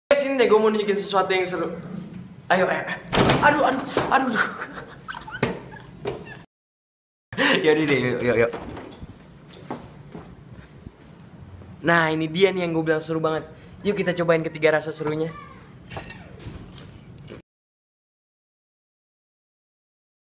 Rassya Cheezfie bareng Shapes Cheezy
Hanya orang-orang yang cool yang berani selfie (bahkan wefie!) dengan pose konyol. They don't need approval. Let's take cheezfie with Shapes Cheezy!